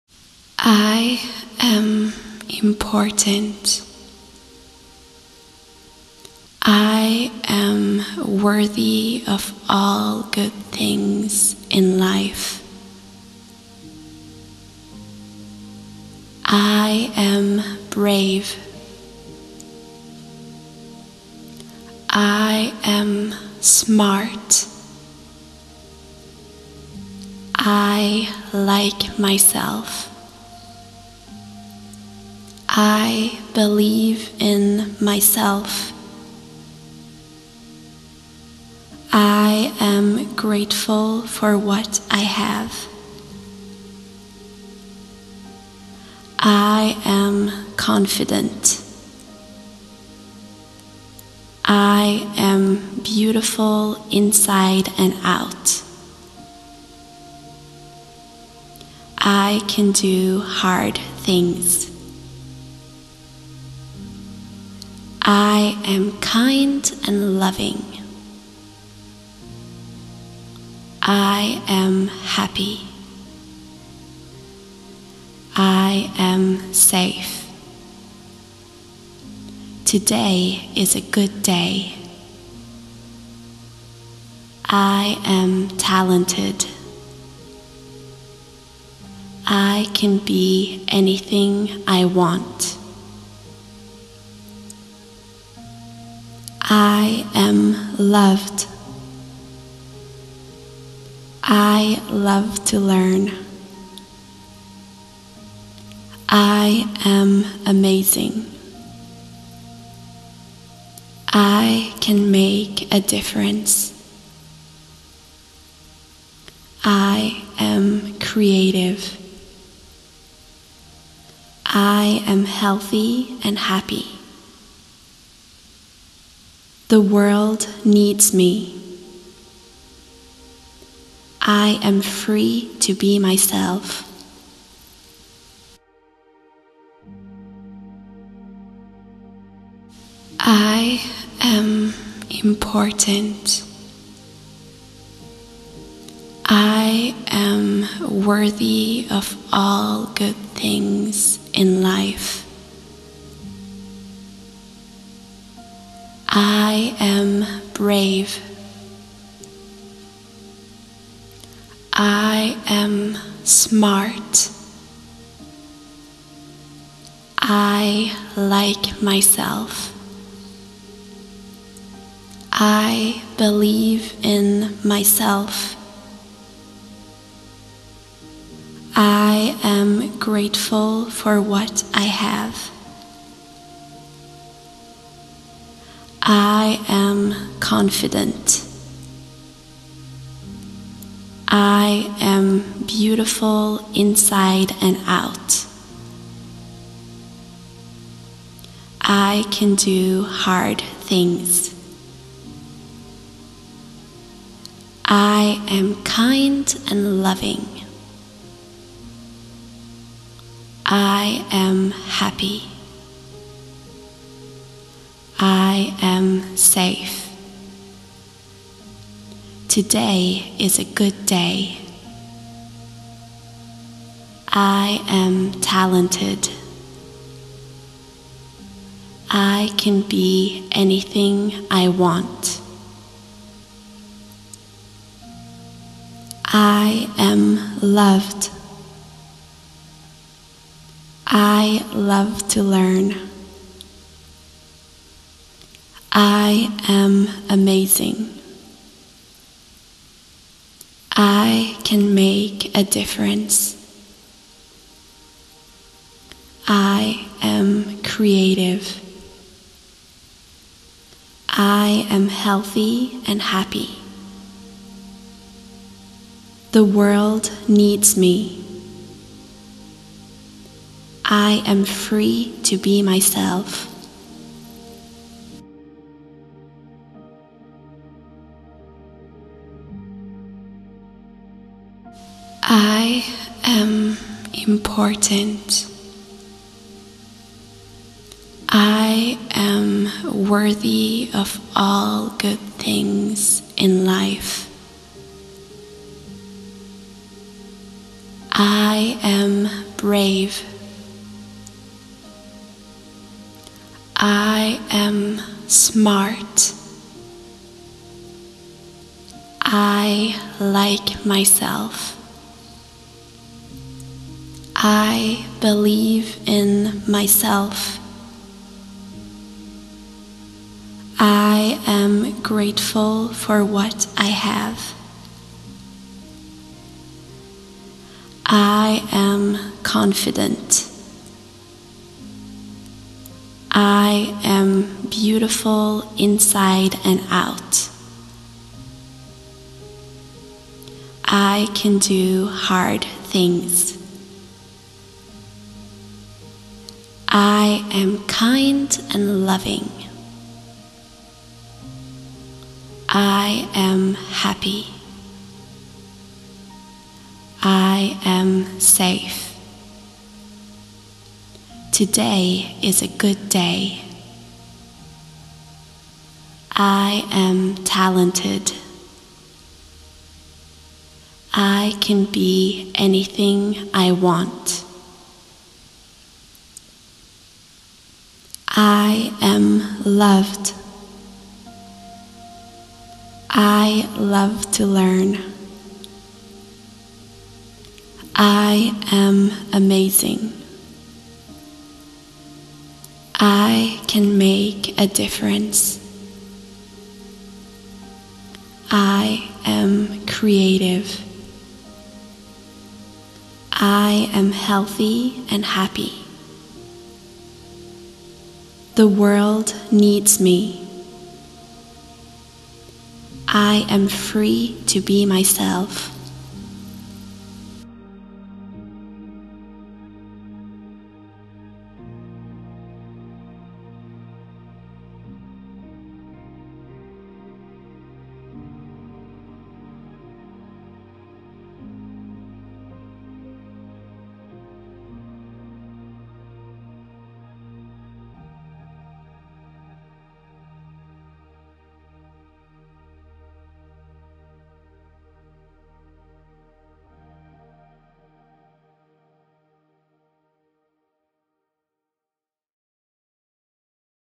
Daily affirmations for children
Hello darlings,

This is daily affirmations you can use daily, for maximum impact, use it for at least 21 days.These are daily affirmations for children 🥰